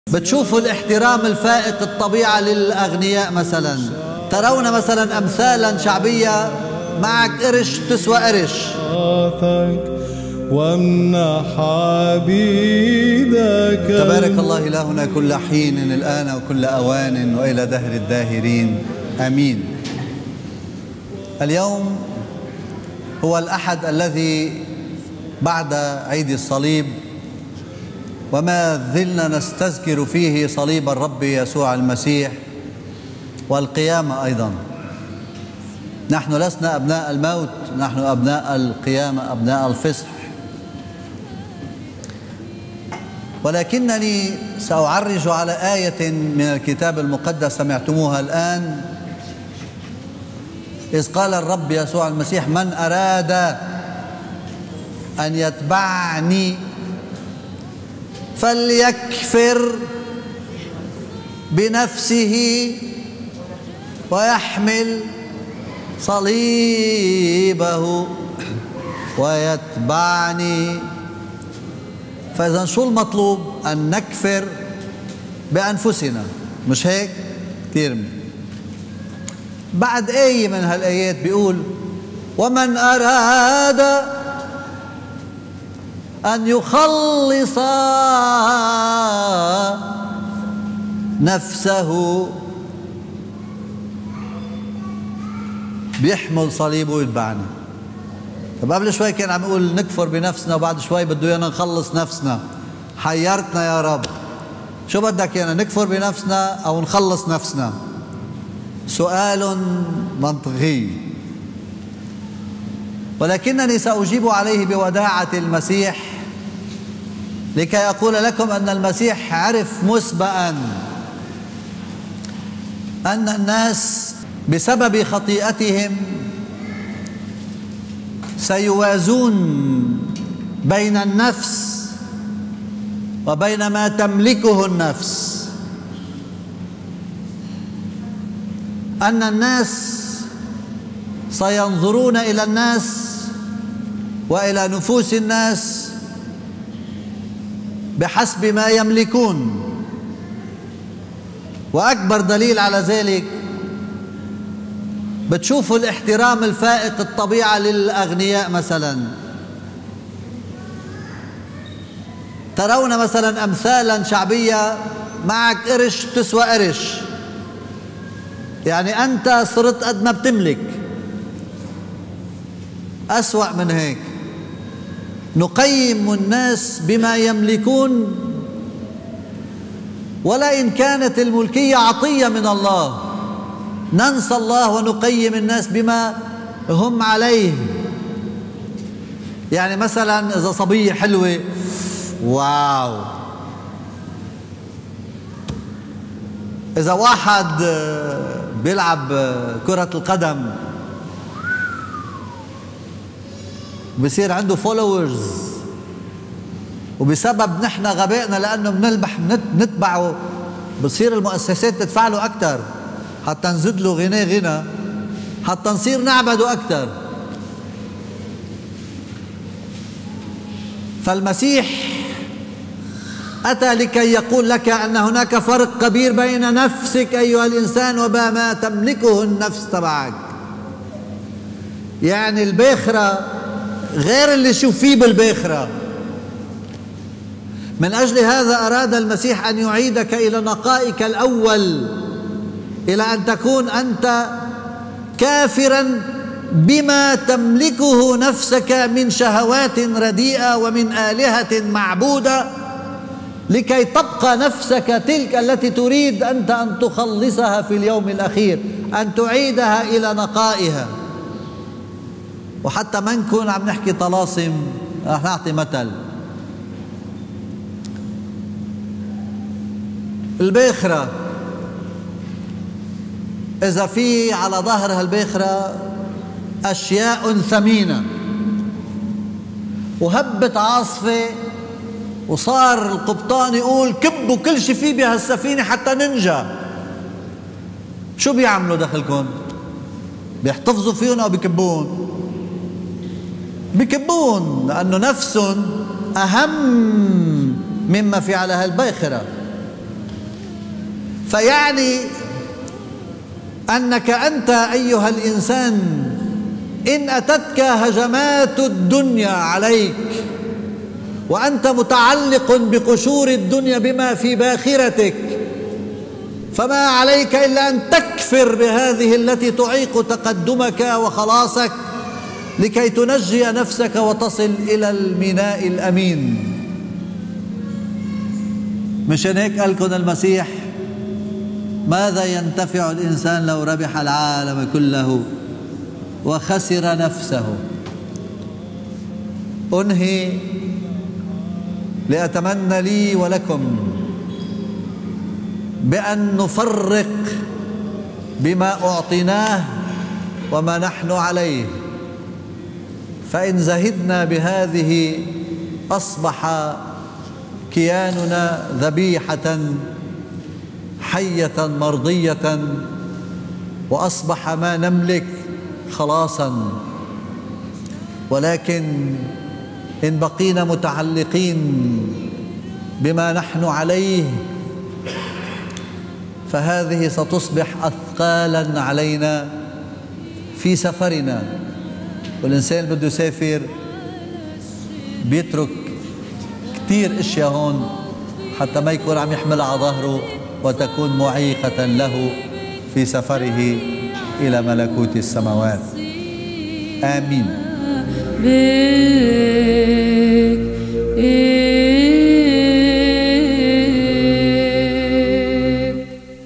فَلْيكفُر بنَفْسَه وليحمل صليبه ويتبعني - مرقس 8: 34-38 - عظة الأب أغابيوس نعوس
نقيّم الناس بما يملكون، ولا إن الكانت الملكيّة عطيّة من الله. ننسى الله ونقيّم الناس بما هم عليه. المسيح أتى لكي يقول لك أن هناك فرق كبير بين نفسك أيّها الإنسان وبما تملكه نفسك
مرقس 8: 34-38
وَدَعَا ٱلْجَمْعَ مَعَ تَلَامِيذِهِ وَقَالَ لَهُمْ: «مَنْ أَرَادَ أَنْ يَأْتِيَ وَرَائِي فَلْيُنْكِرْ نَفْسَهُ وَيَحْمِلْ صَلِيبَهُ وَيَتْبَعْنِي. 35فَإِنَّ مَنْ أَرَادَ أَنْ يُخَلِّصَ نَفْسَهُ يُهْلِكُهَا، وَمَنْ يُهْلِكُ نَفْسَهُ مِنْ أَجْلِي وَمِنْ أَجْلِ ٱلْإِنْجِيلِ فَهُوَ يُخَلِّصُهَا. 36لِأَنَّهُ مَاذَا يَنْتَفِعُ ٱلْإِنْسَانُ لَوْ رَبِحَ ٱلْعَالَمَ كُلَّهُ وَخَسِرَ نَفْسَهُ؟ 37أَوْ مَاذَا يُعْطِي ٱلْإِنْسَانُ فِدَاءً عَنْ نَفْسِهِ؟ 38لِأَنَّ مَنِ ٱسْتَحَى بِي وَبِكَلَامِي فِي هَذَا ٱلْجِيلِ ٱلْفَاسِقِ ٱلْخَاطِئِ، فَإِنَّ ٱبْنَ ٱلْإِنْسَانِ يَسْتَحِي بِهِ مَتَى جَاءَ بِمَجْدِ أَبِيهِ مَعَ ٱلْمَلَائِكَةِ ٱلْقِدِّيسِينَ».